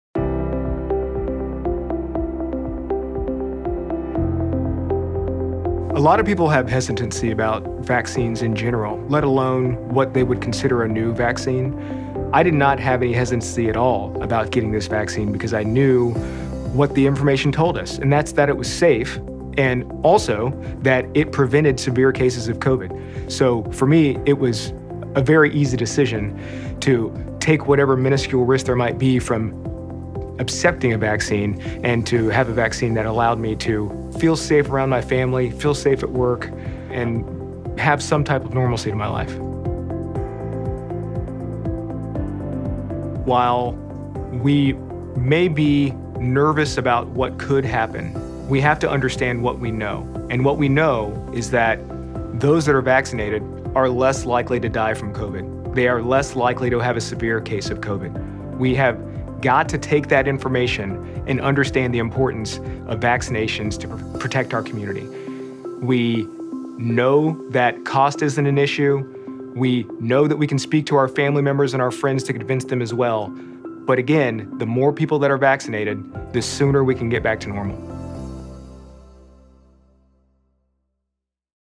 Mayo Clinic Insights: Why receiving the COVID-19 vaccine is beneficial
Dr. Ivan Porter II explains how the benefits of the COVID-19 vaccine outweigh the risks of not being vaccinated. For more up to date information about the COVID-19 vaccine,